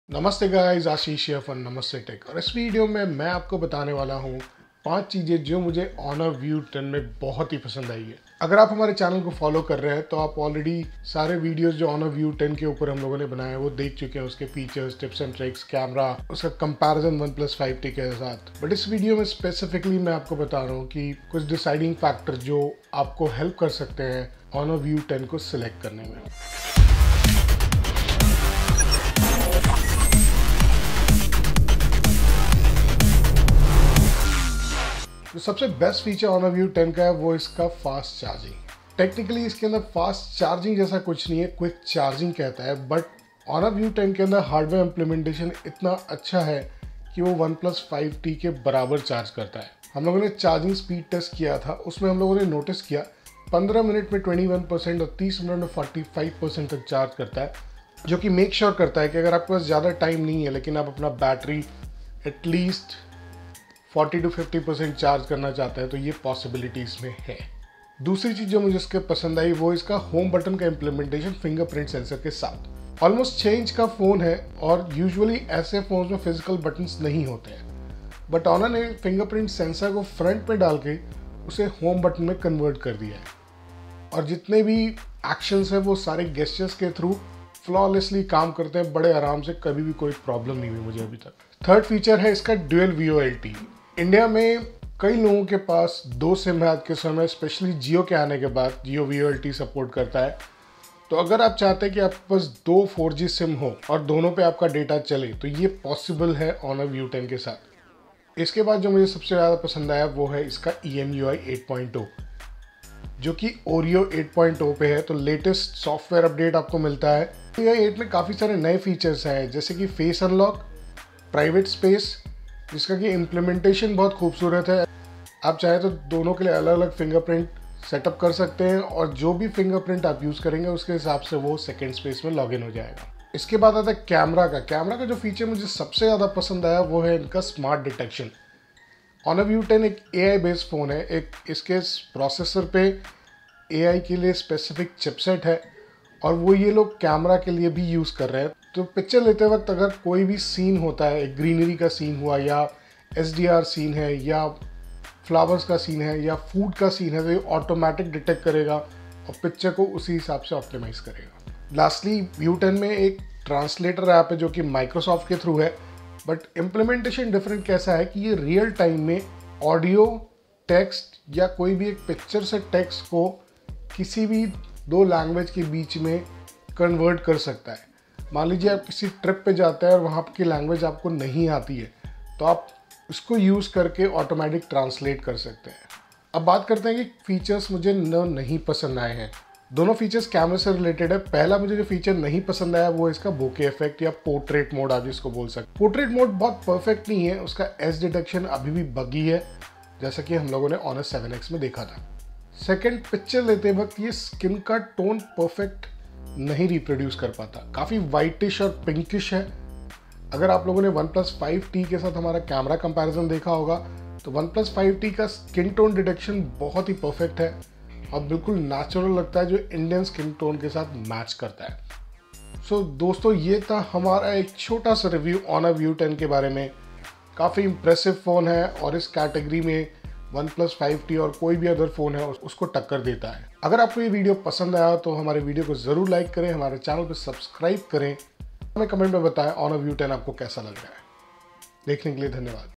Honor View 10 Review: What we loved about this phone
Check out what we loved, and we didn't in this review of Honor View 10